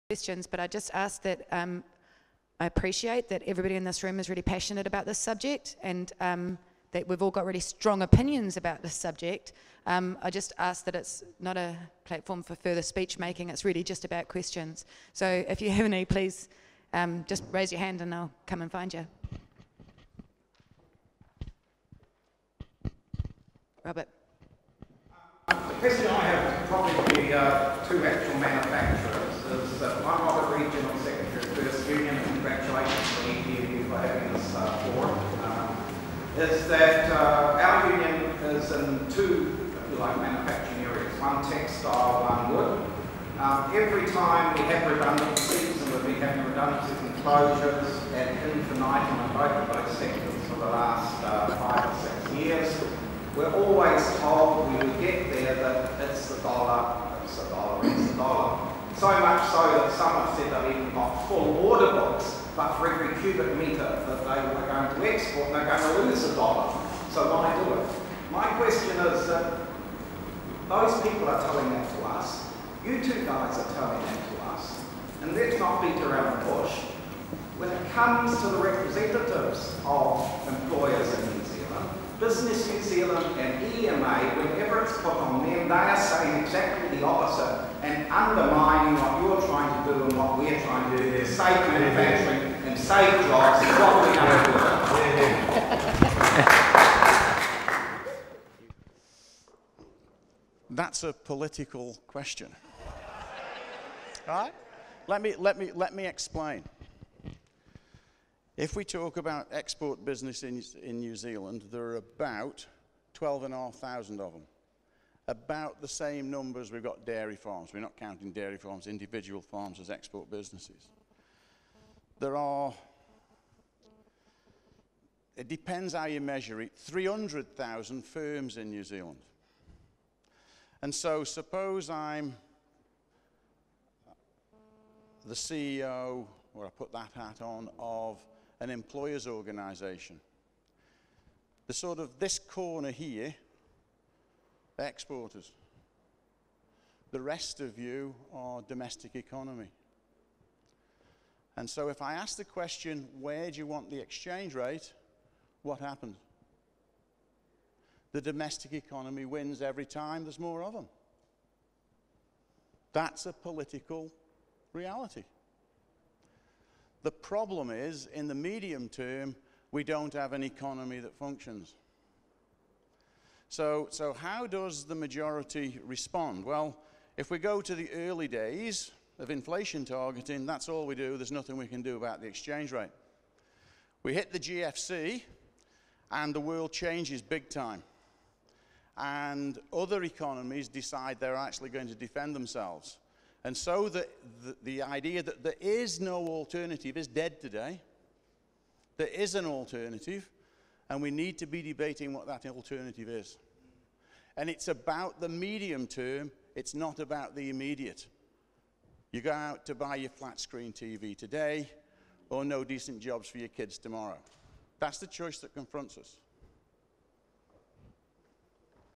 EPMU Jobs Crisis Summit (Q&A Part 1) - Auckland 12 October 2012
EPMU Jobs Crisis Summit held in Auckland on 12 October 2012. MPs and business people discussed economic policy settings in the context of the continuing loss of NZ jobs.